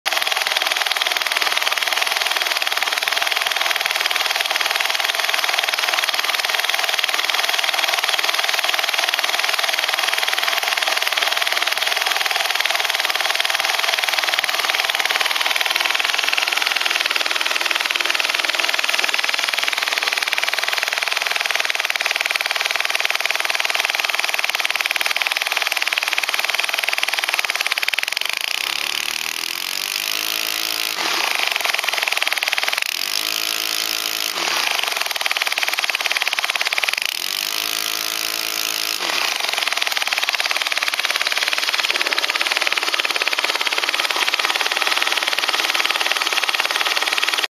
34cc 4 stroke grass trimmer converted to R/C
This is a test unit. With 18x8, the max rpm is 6700 after one hour of test run. The specialiest design is the carburator, exclusively without low end and hign end needles.